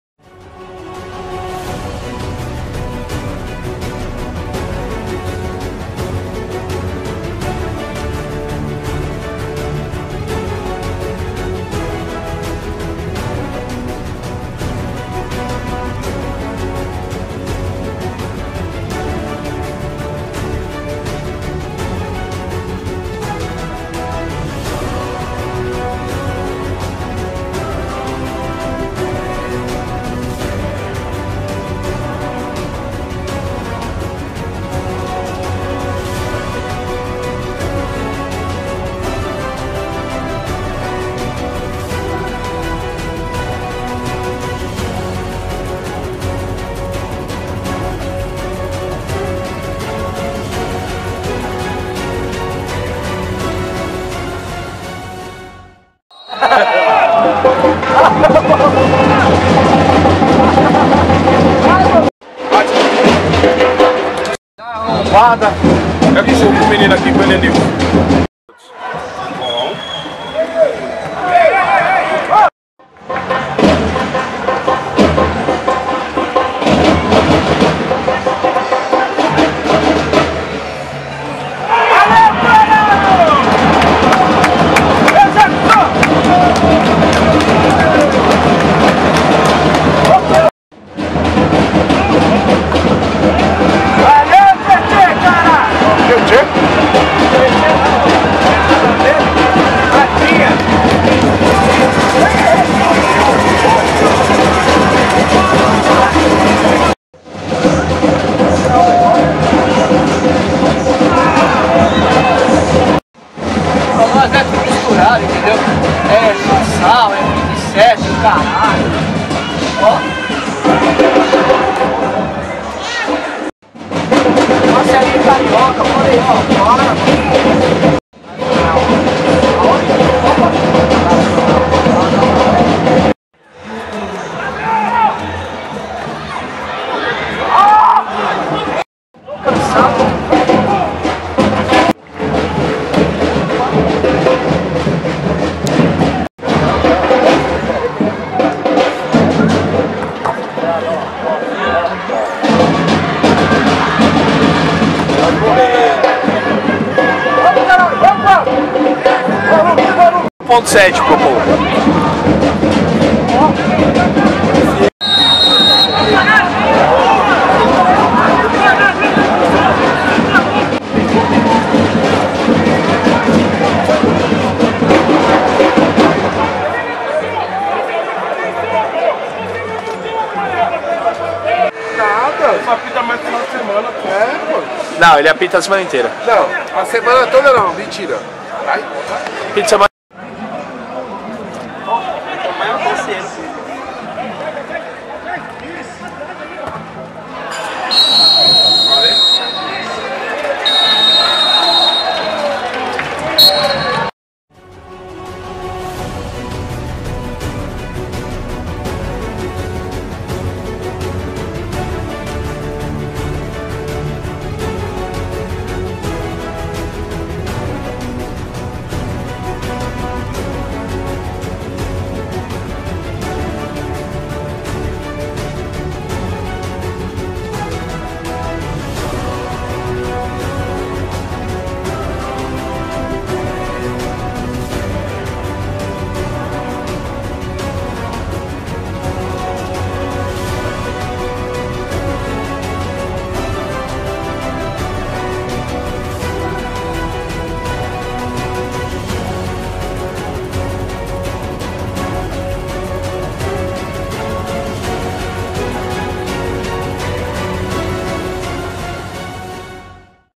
Futebol é Coisa Séria |Copa da Paz 2019| Só Kachaça x Garotos
O Só Kachaça superou a expulsão do seu goleiro (tendo que improvisar um jogador de linha no gol) e venceu o Garotos na estreia de ambos na Copa da Paz 2019